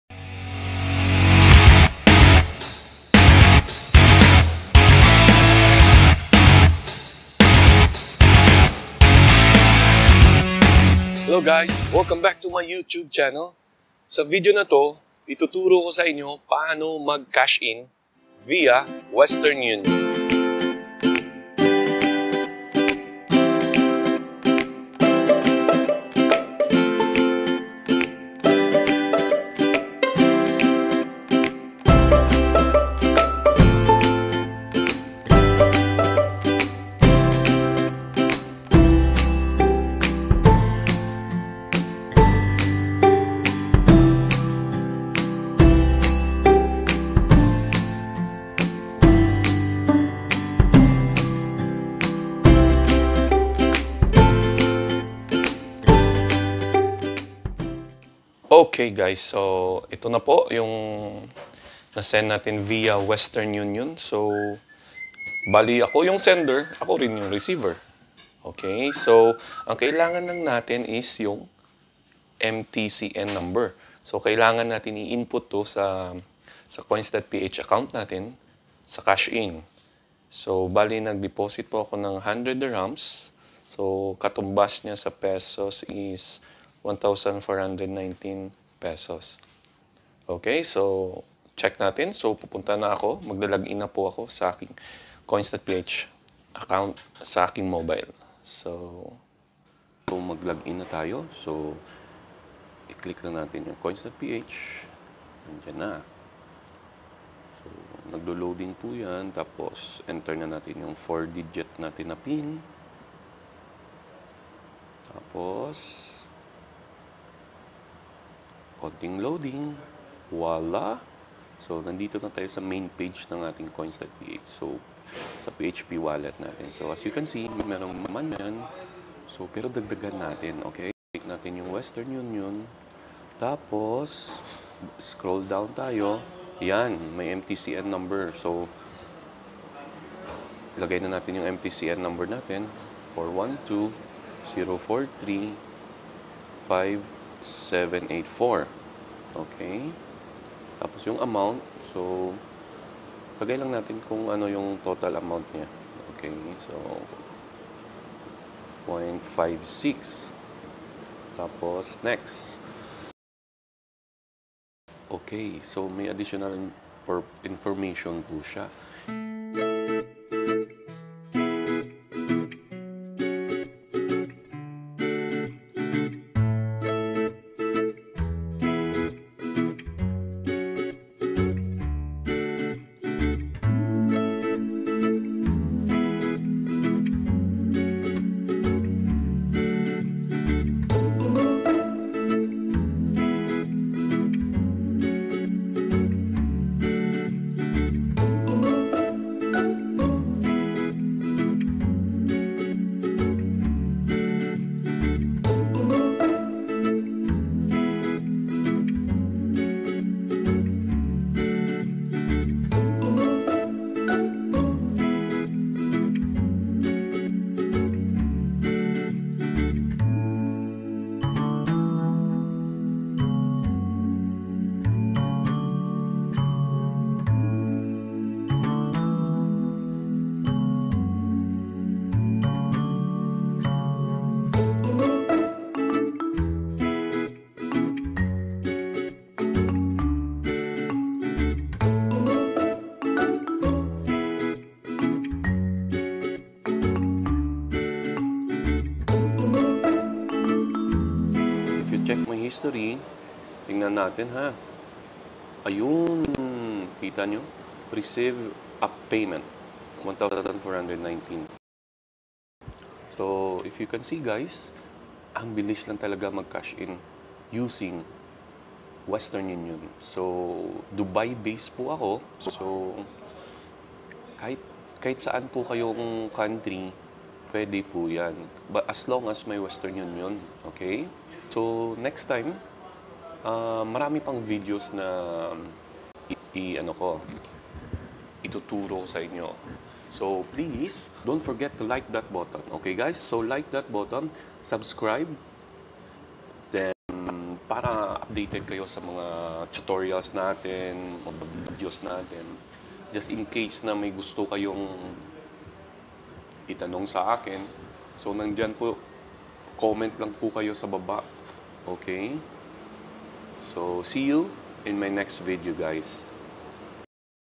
Paano mag cash in sa coins.ph wallet step by step via western union
Paano mag cash in sa coins.ph wallet step by step po ito
sundin nyo lang at ako mismo ng try niito at succesfull.

CREATE NEW ACCOUNT IN COINS.PH

Maraming Salamat

Please pa like and subscirbe.